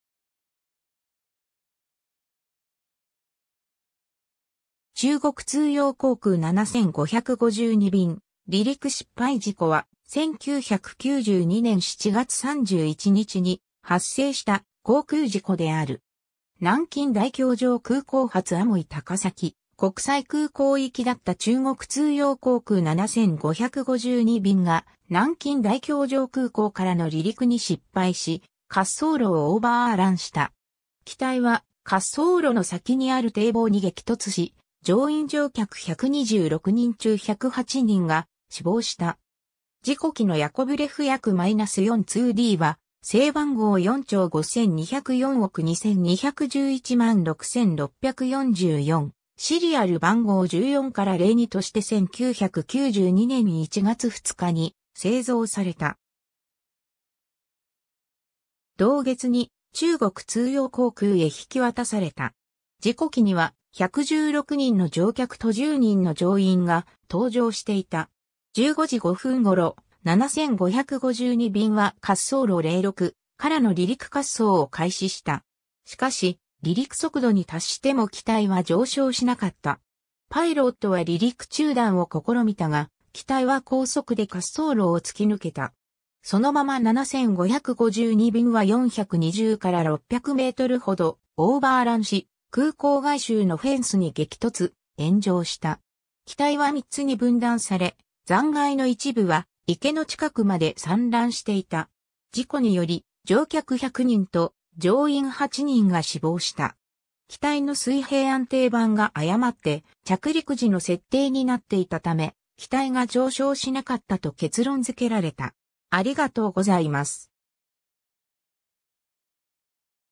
中国通用航空7552便離陸失敗事故
Yak-42による航空事故
中国通用航空7552便離陸失敗事故は、1992年7月31日に発生した航空事故である。
南京大教場空港発廈門高崎国際空港行きだった中国通用航空7552便（ヤコヴレフ Yak-42D）が、南京大教場空港からの離陸に失敗し滑走路をオーバーランした。
機体は、滑走路の先にある堤防に激突し、乗員乗客126人中108人が死亡した。
 事故機のヤコヴレフ Yak-42D（B-2755）は、製番号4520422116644、シリアル番号14-02として1992年1月2日に製造された。
同月に、中国通用航空へ引き渡された。
 事故機には、116人の乗客と10人の乗員が搭乗していた。
15時05分頃、7552便は滑走路06からの離陸滑走を開始した。
しかし、離陸速度に達しても機体は上昇しなかった。
パイロットは離陸中断を試みたが、機体は高速で滑走路を突き抜けた。
そのまま7552便は420～600mほどオーバーランし、空港外周のフェンスに激突、炎上した。
機体は3つに分断され、残骸の一部は池の近くまで散乱していた。
事故に因り、乗客100人と乗員8人が死亡した。
 機体の水平安定板が誤って着陸時の設定になっていたため、機体が上昇しなかったと結論付けられた。